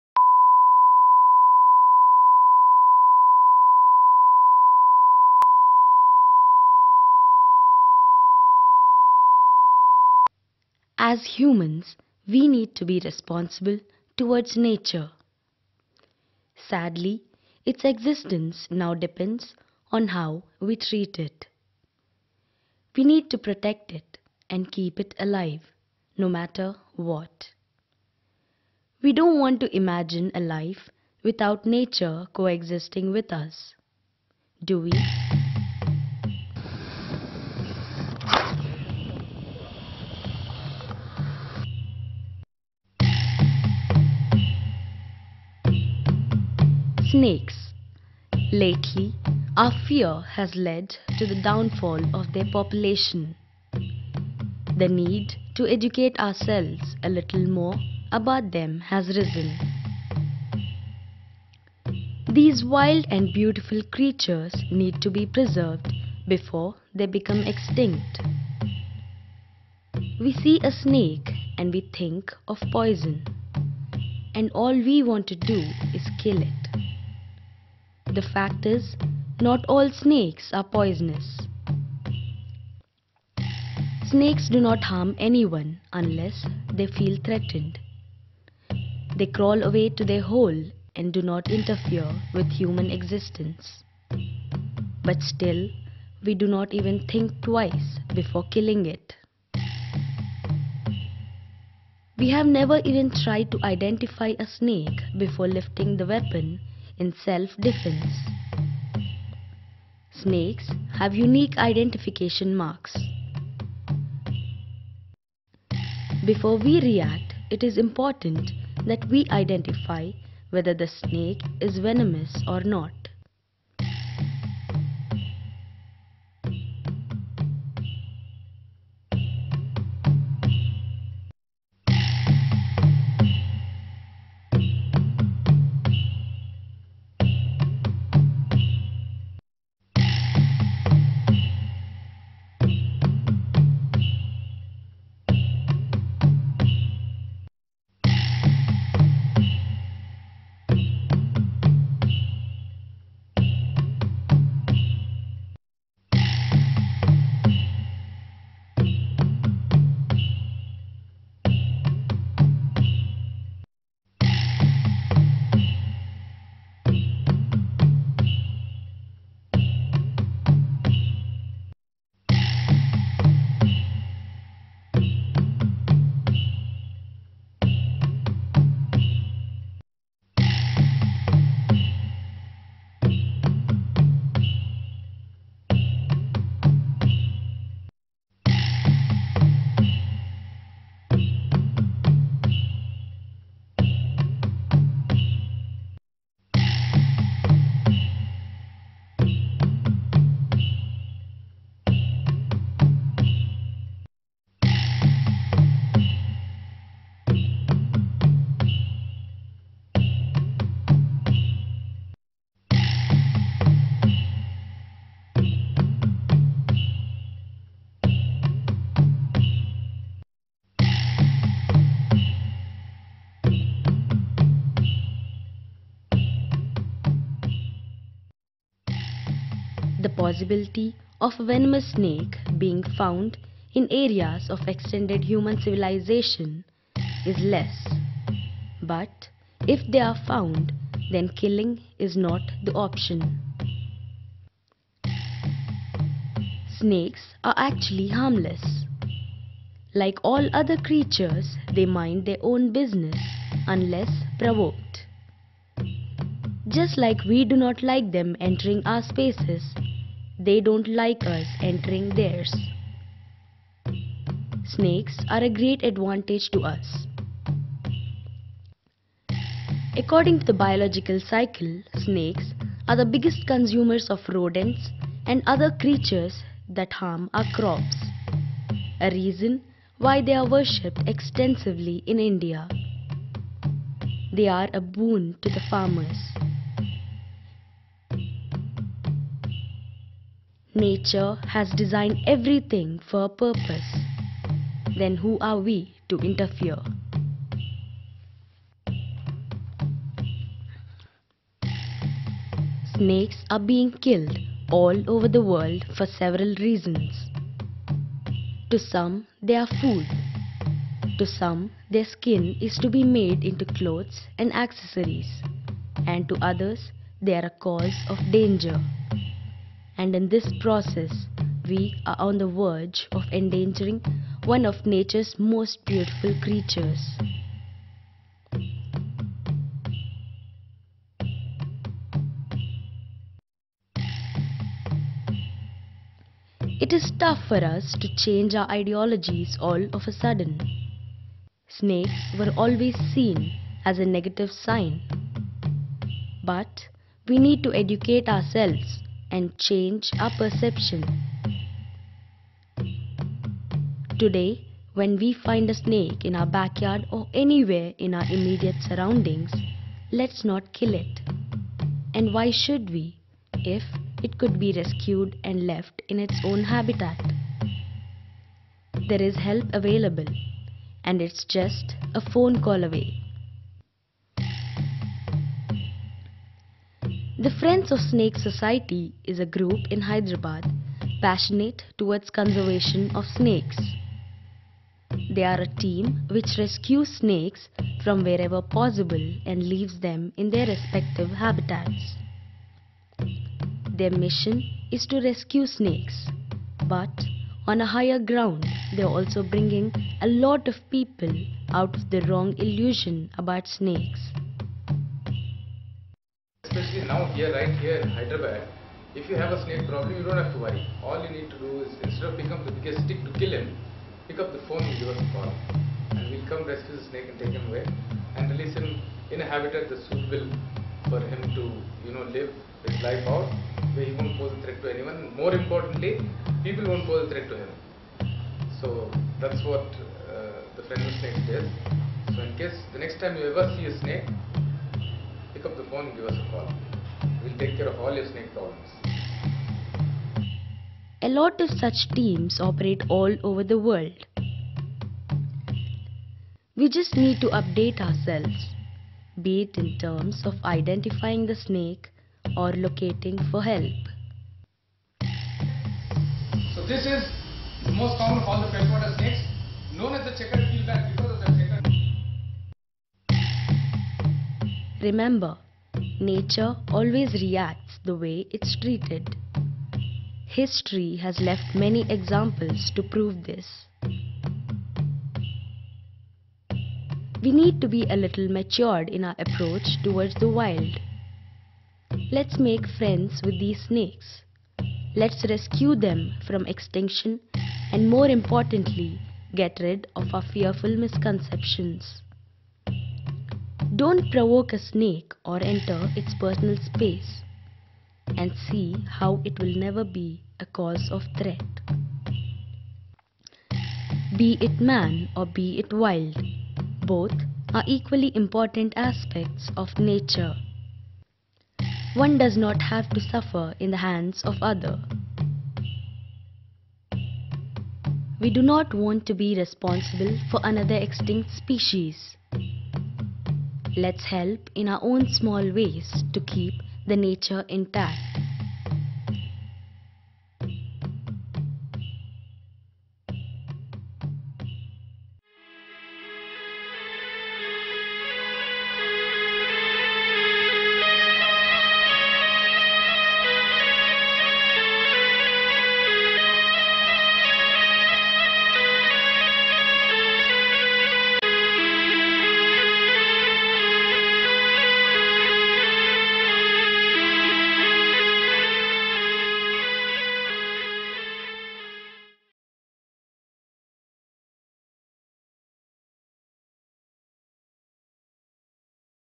Creeping Extinction..!!
Snakes are beautiful creatures, vital for a balance in the ecosystem..the rate at which they are being wiped out is alarming...a small awareness clip and an effort to save them by FRIENDS OF SNAKES ASSOCIATION, hyderabad..
we are just a call away..

P.S. the video has a slow start..plz b patient and watch it fully..!